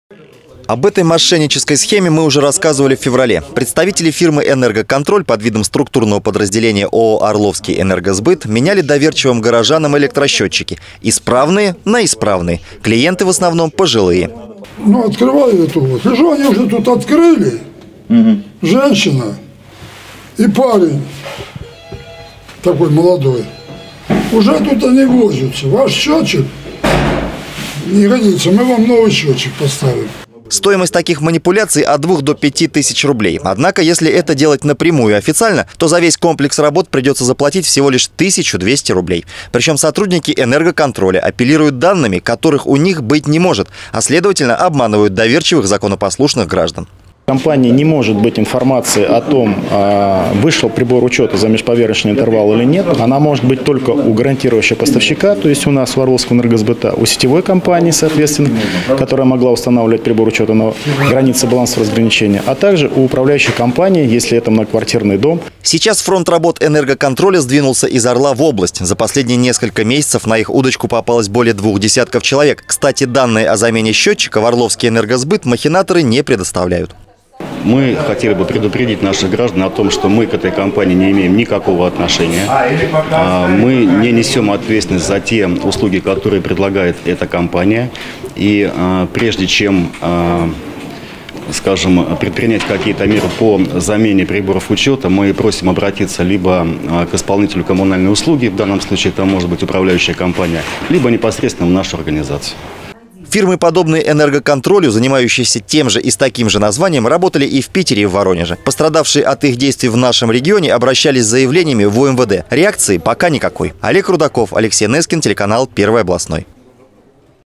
В Орле раскрыта новая схема мошенничества с электрическими счетчиками
Более 100 орловчан за 3 месяца попались на уловку нечистых на руку дельцов. Некая фирма меняла якобы устаревшие электрические счетчики на новые. По ценам в 3-4 раза выше официальных. За отказ менять счетчик грозили санкциями, при этом прикрывались серьезными организациями.